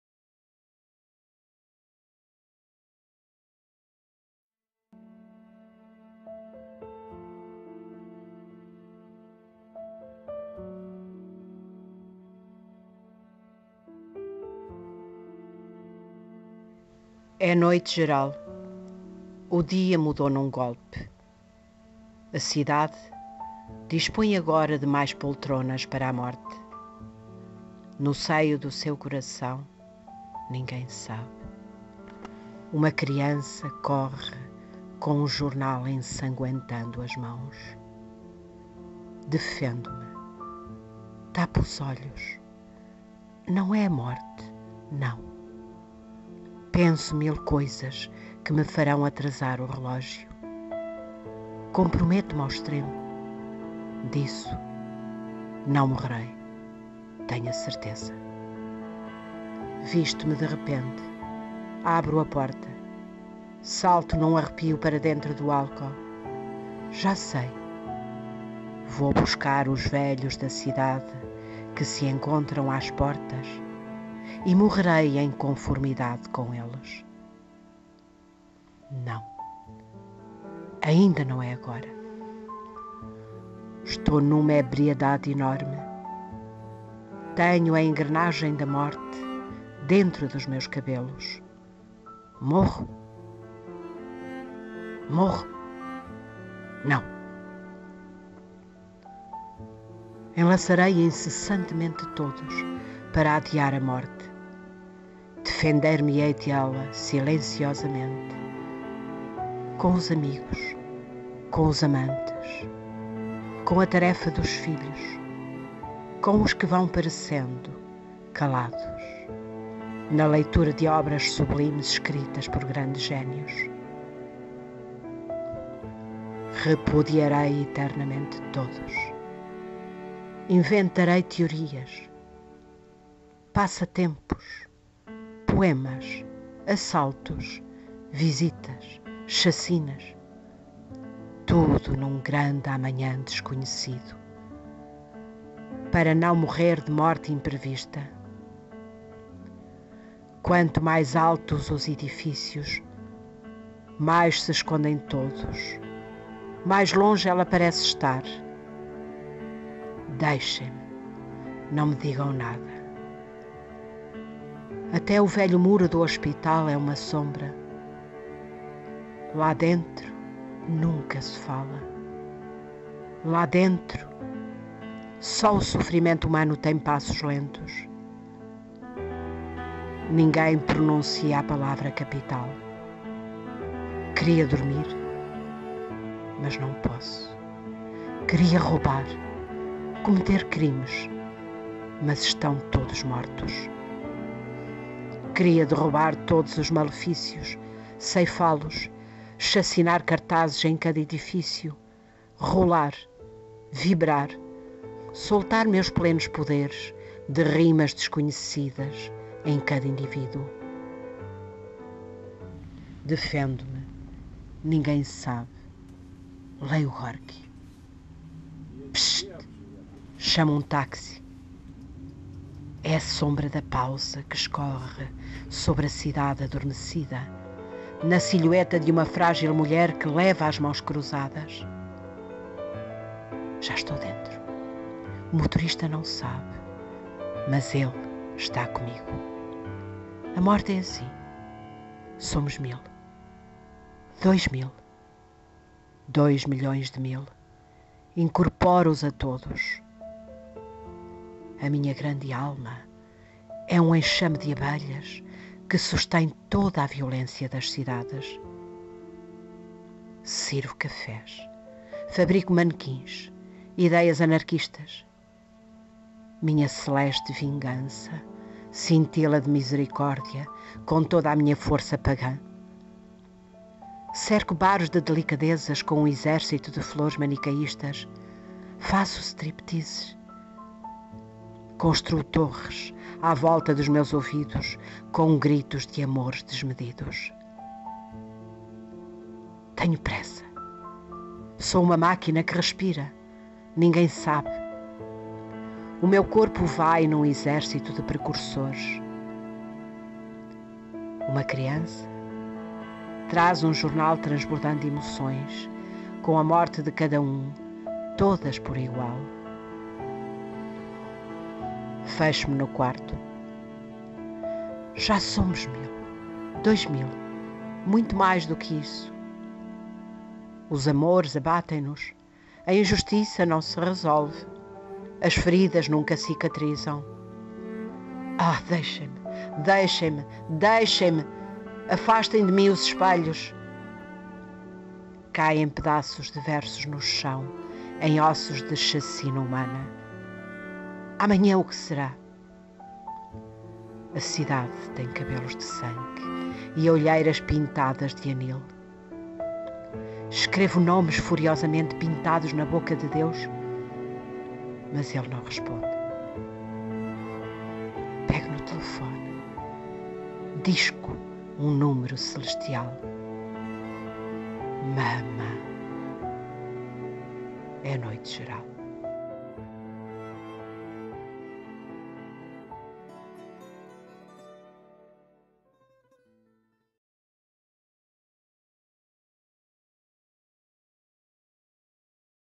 É NOITE GERAL
´ É NOITE GERAL '  - escrito em Julho de 2007 e inserido no livro "A MAMÃ POR CIMA DOS TELHADOS E O MEU AMOR", Maria Azenha, Editora Urutau, 2019
Poema e voz  de Maria Azenha
Uma realização: CaNaL ArTe 2020